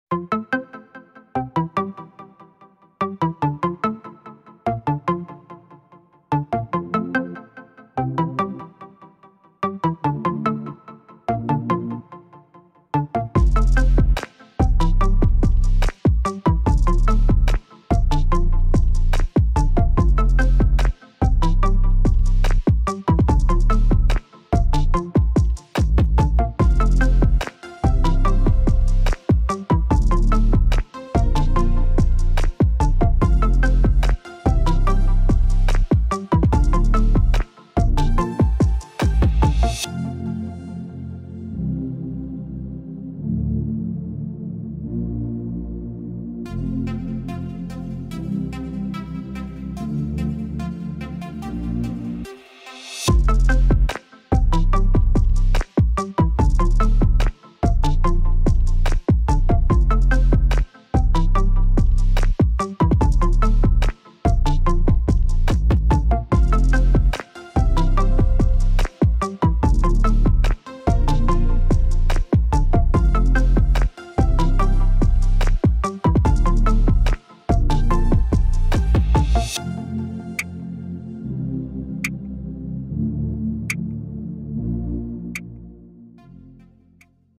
How to Draw a Cube | Solid Vs Frame
Easy step-by-step drawing of a solid cube and a cube frame.